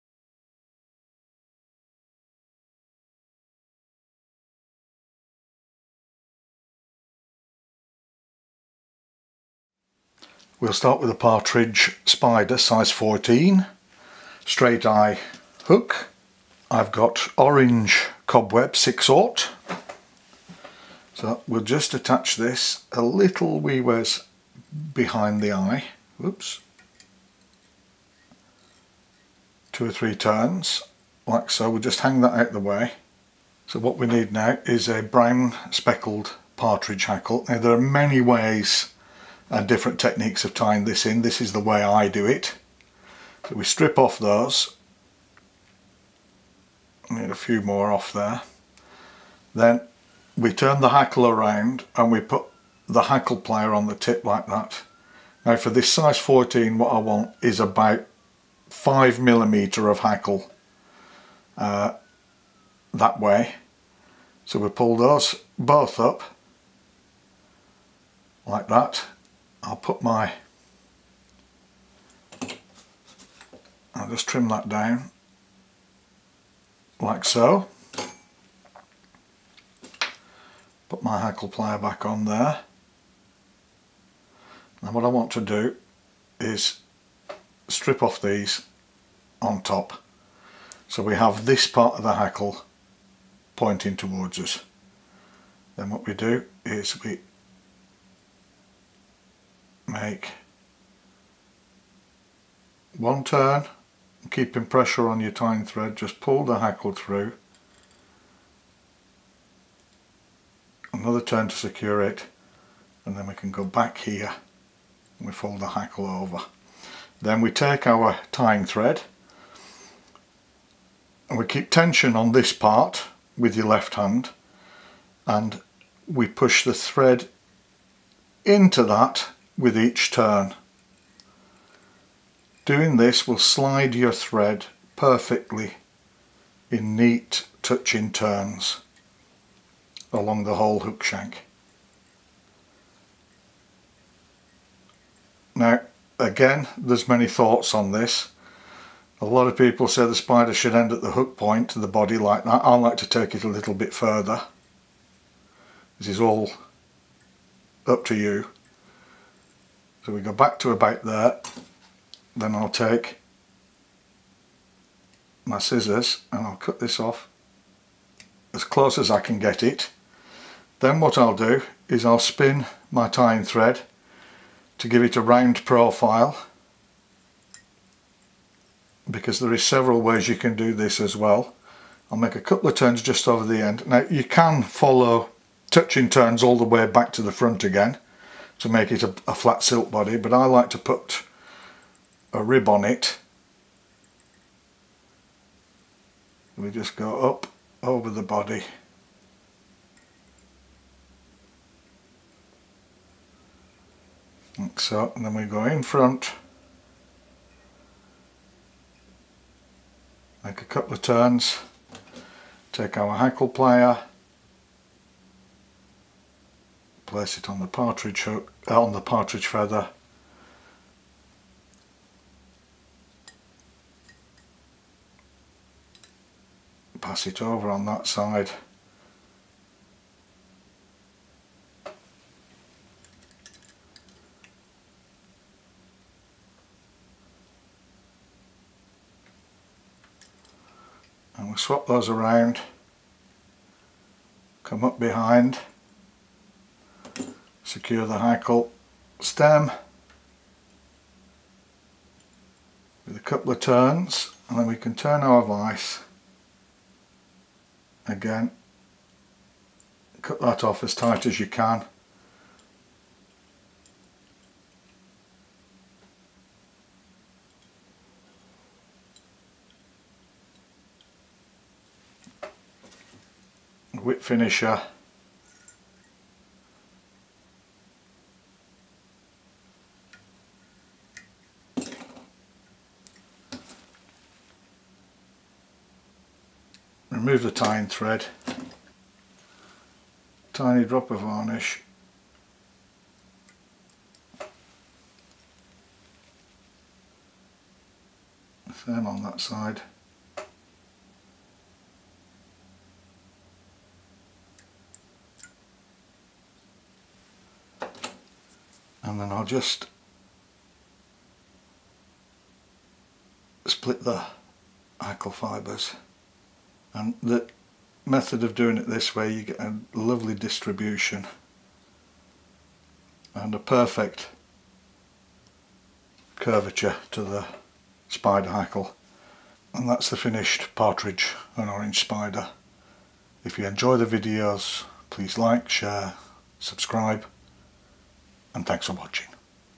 Fly Tying a Partridge & orange spider with Barry Ord Clarke
This is my take on one of the all time classic soft hackle patterns that in recent years has experienced a comeback.